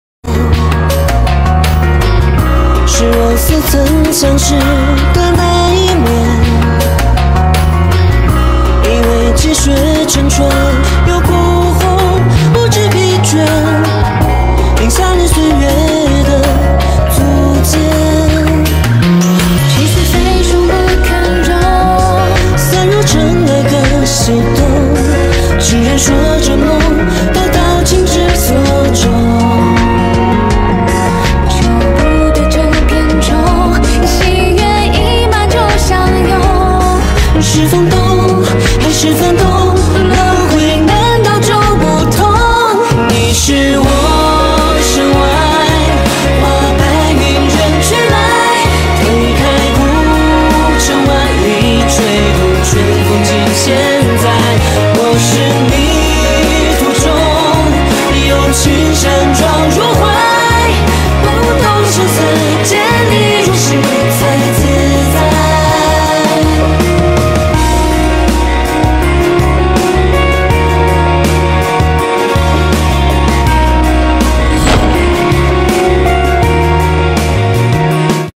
Neo Hou 侯明昊 ในการถ่ายทำซีรีย์ล่าหัวใจมังกร (Back from the brink / 护心 )
Cr. clip from weibo 永远昕动的猕猴桃
เรื่องนี้จะปิดกล้องในเดือน ธ.ค ออนแอร์ปี 2022 จำนวน 40 ตอนค่ะ